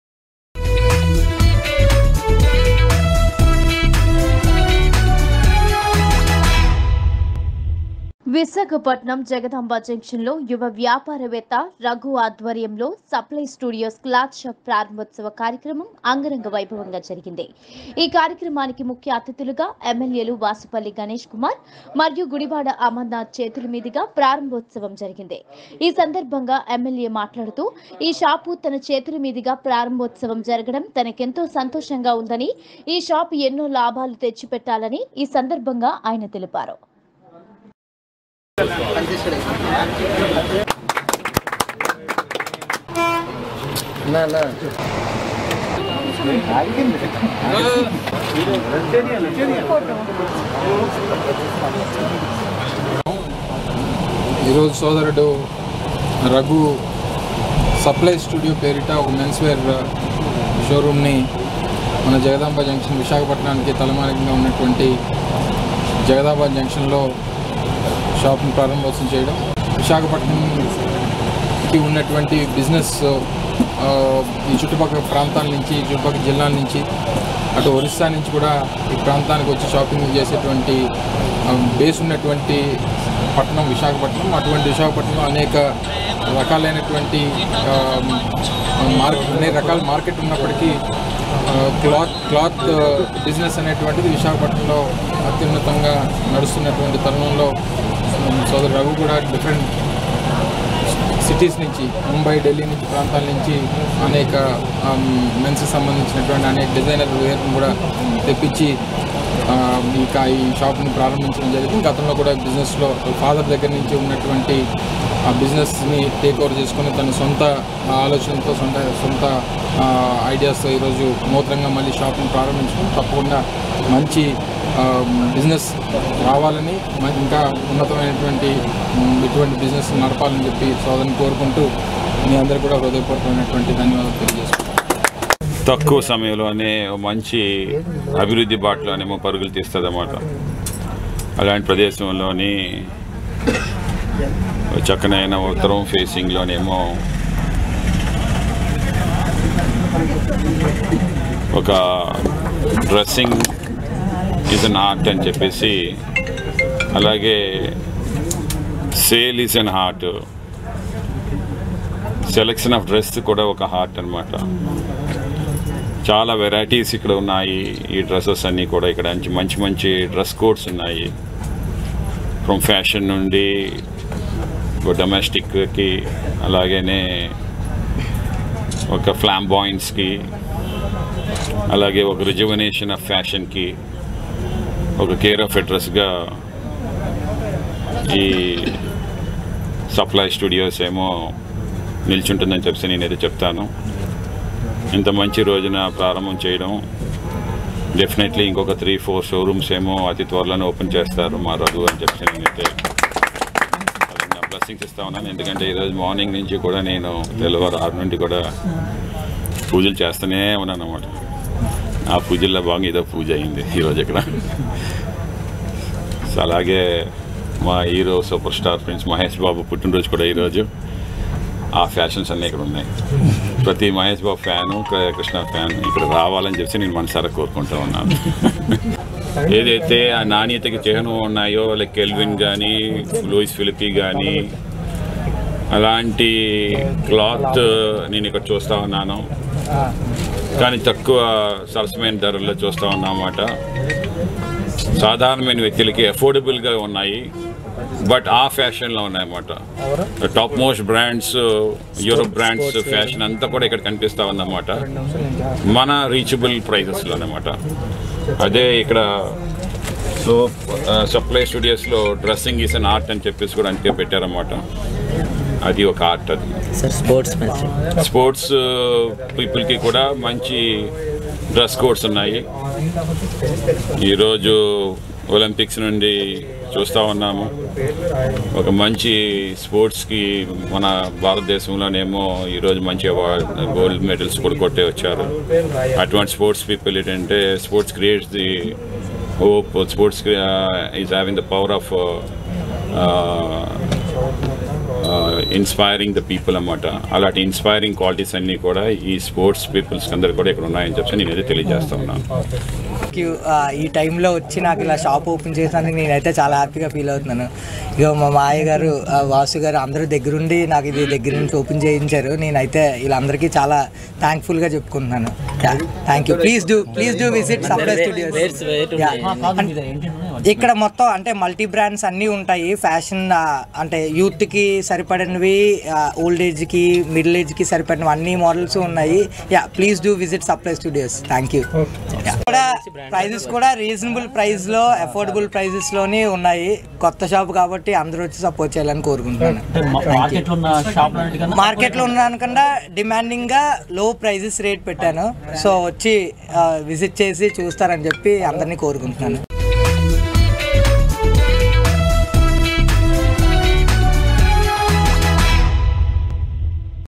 సప్లై స్టూడియోస్ క్లాత్ షోర్ రూమ్ ప్రారంభించిన MLA వాసుపల్లి గణేష్..
@truthnewstelugu సప్లై స్టూడియోస్ క్లాత్ షోర్ రూమ్ ప్రారంభించిన MLA వాసుపల్లి గణేష్..